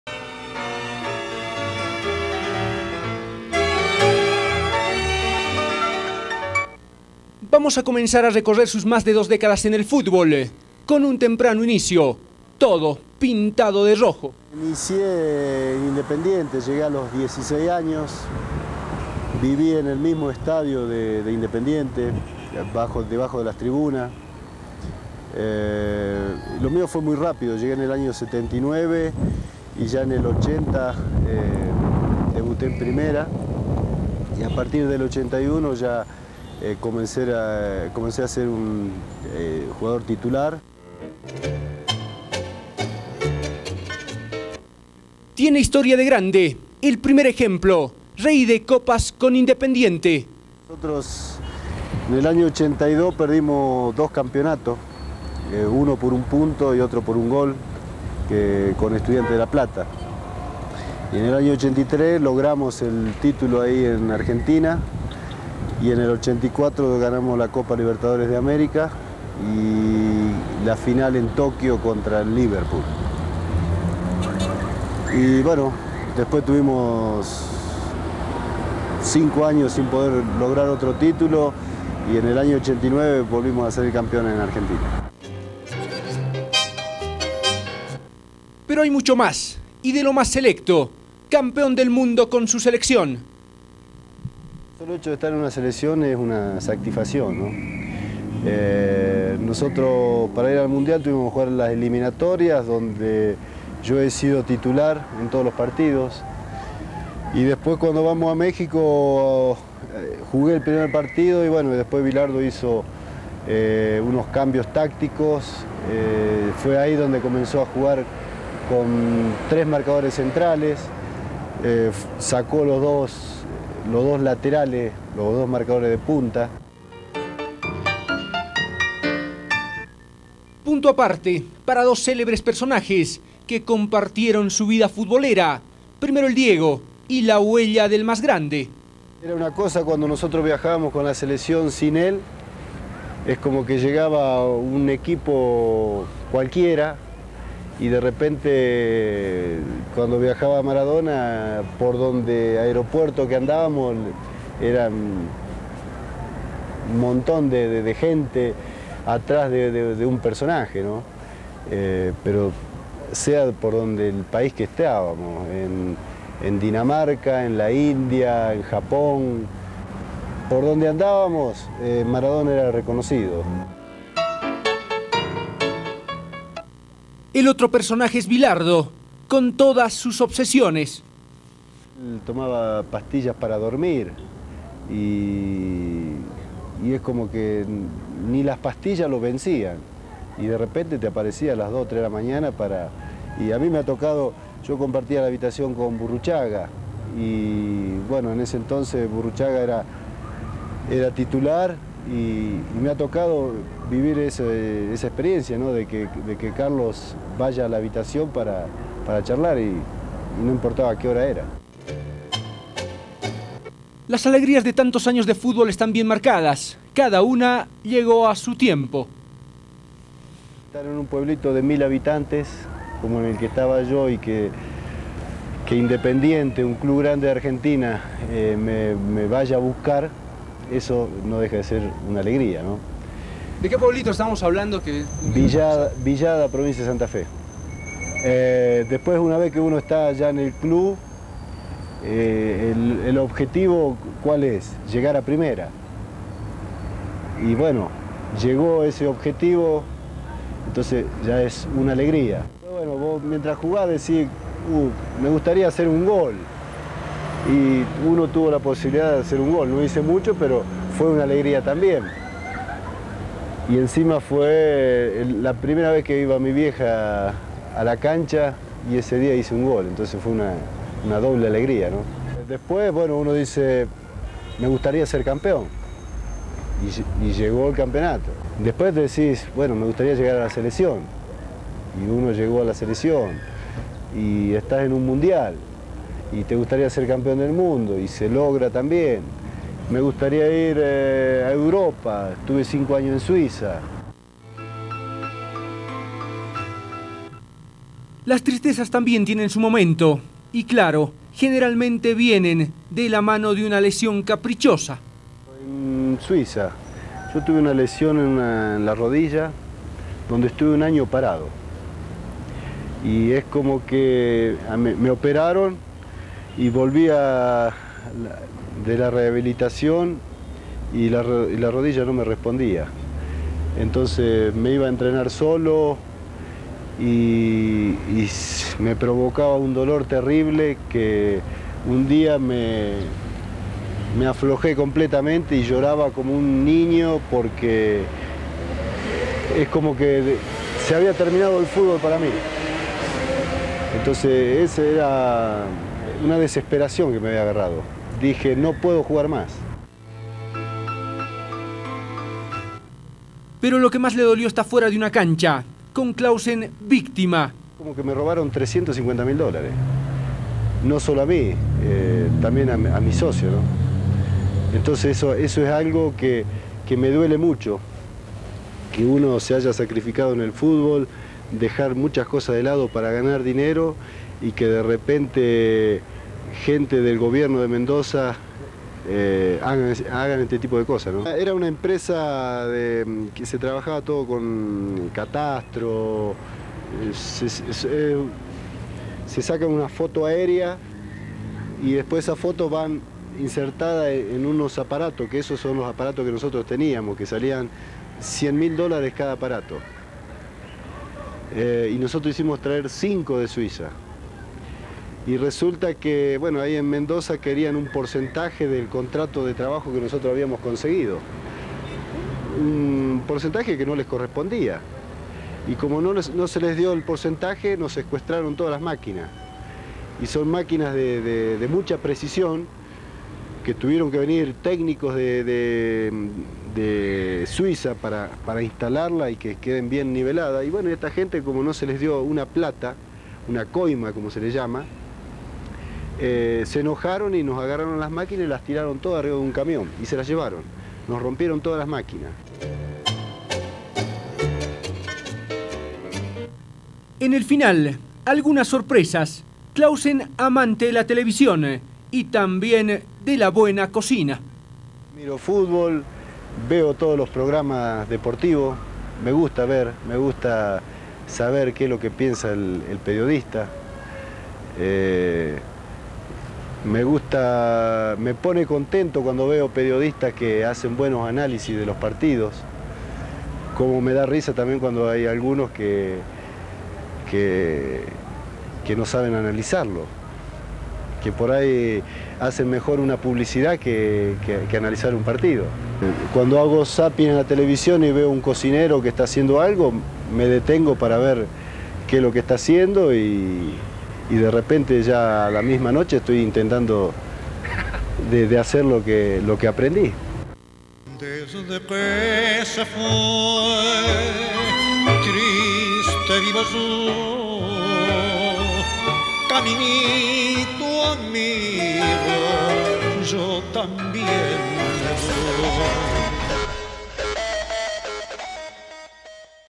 Nestor Clausen - Ex futbolista argentino | El elegido
Que  jugó en Independiente de Argentina y fue campeón del Mundo con su selección. Como entrenador dirigió Oriente, The Strongest, Bolívar, Wilstermann,Blooming , San José y Real Santa Cruz.